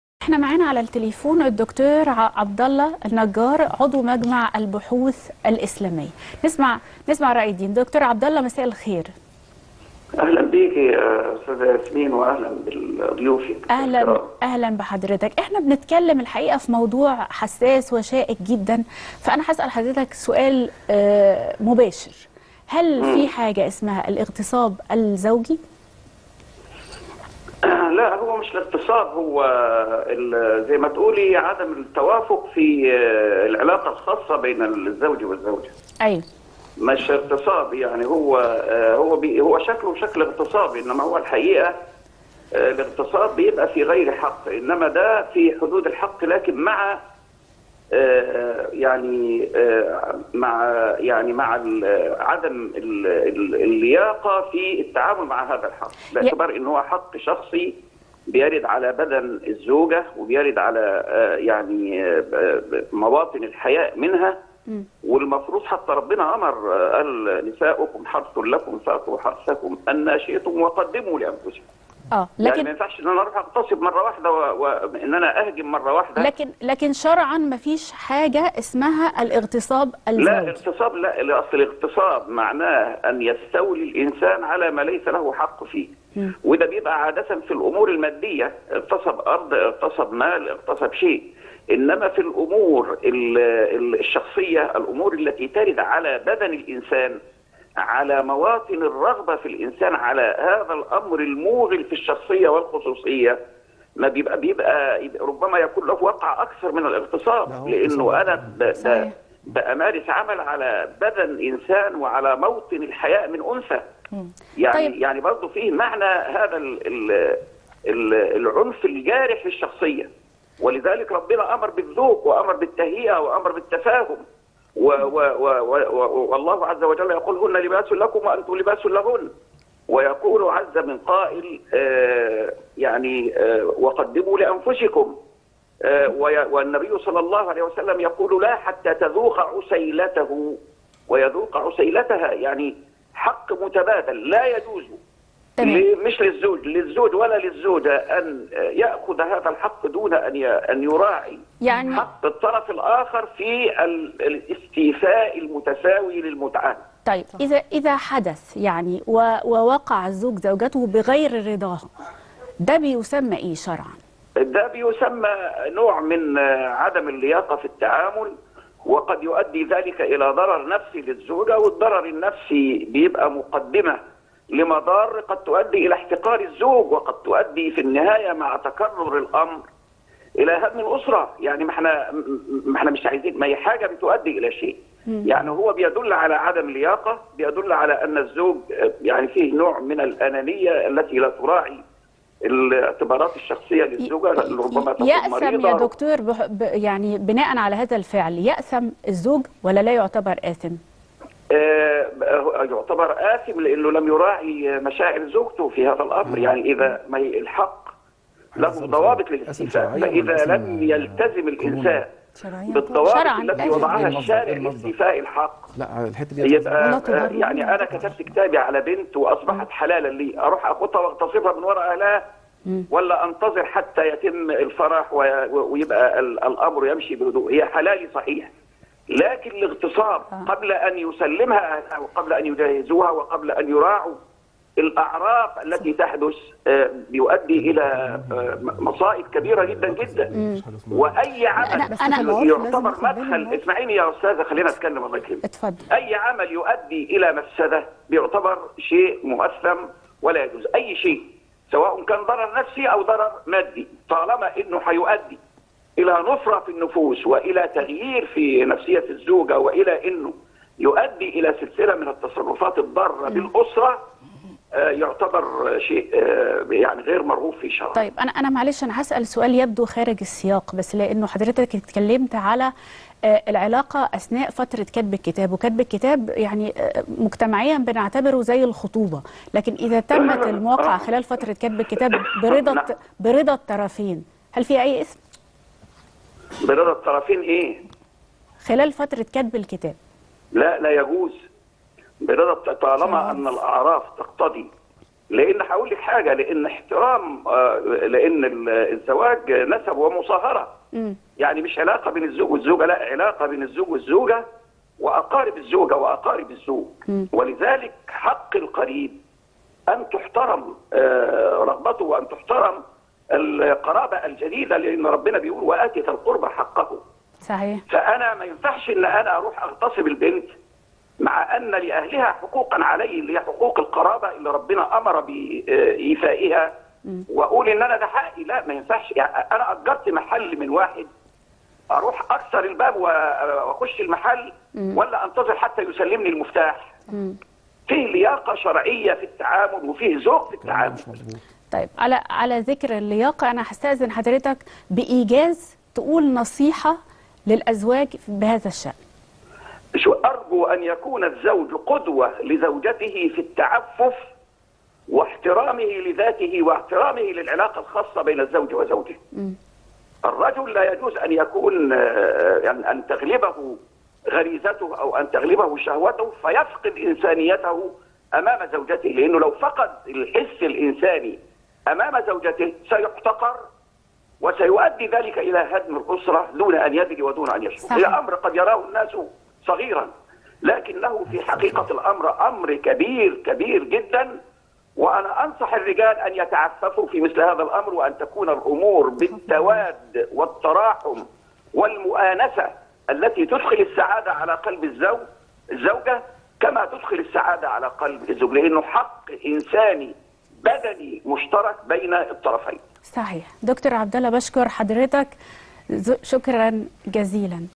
حكم الشرع في إقامة علاقة جنسية بين المخطوبين بعد كتب الكتاب..حدود الخطوبة
ونقول كمان | حكم الشرع في إقامة علاقة جنسية بين المخطوبين بعد كتب الكتاب..حدود الخطوبة